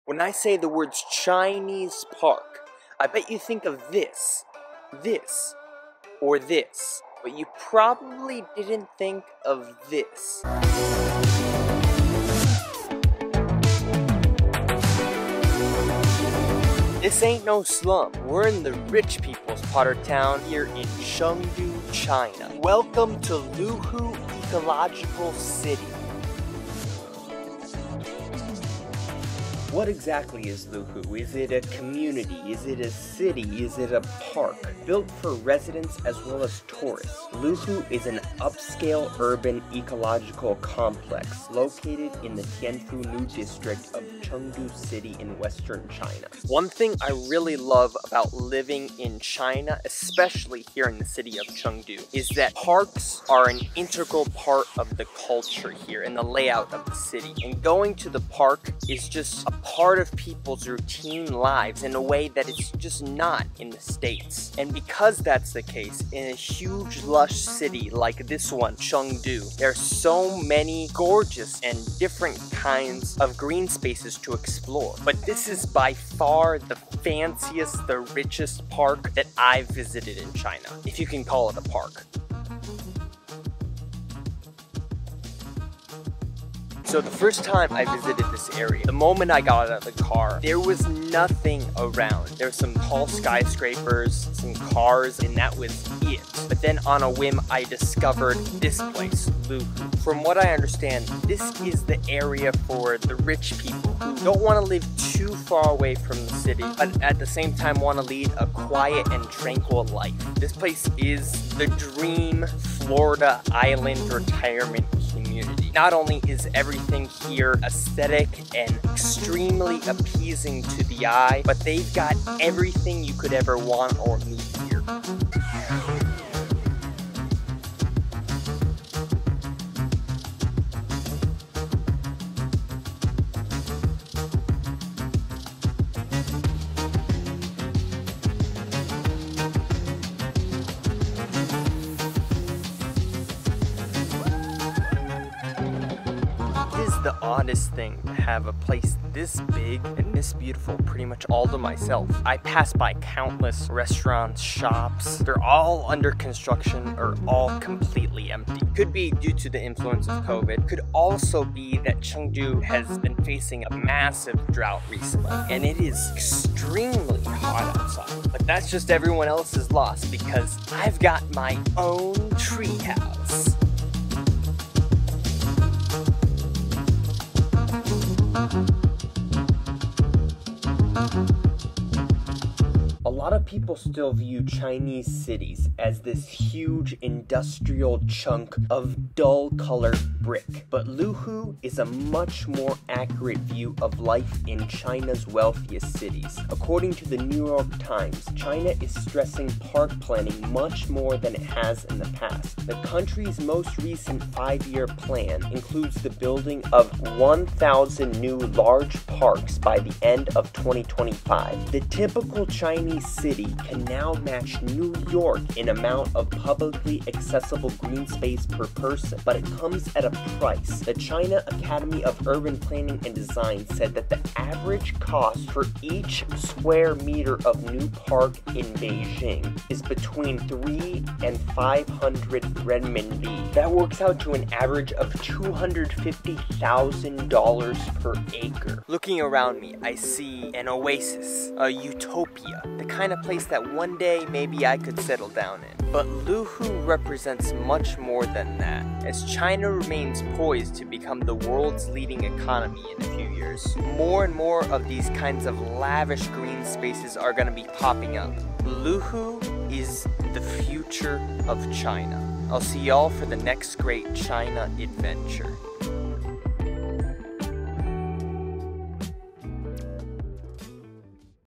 A Day Exploring China's Ecological City (Luhu)
In this vlog, I spent a day exploring China's luxurious Luhu Ecological City in Chengdu, China (also known as Luxe Lakes). Luhu is a high-end urban ecological building complex integrating housing, tourism, leisure, and entertainment. It's a lush, green piece of heaven full of fancy bars, shops, cafes, sporting venues and an animal farm.

Check out the New York Times article about China's park planning

Video Clip Credits:
Edmond Dantès
tom fisk
Caleb Oquendo
Engin Akyurt
WOO WOO
Magda Ehlers
RODNAE Productions
Distill
YAUHENI ZAITSAU

Chapters
0:00 Intro
0:29 What is Luhu?
2:05 Exploring the Ecological City
3:02 This is the Future of China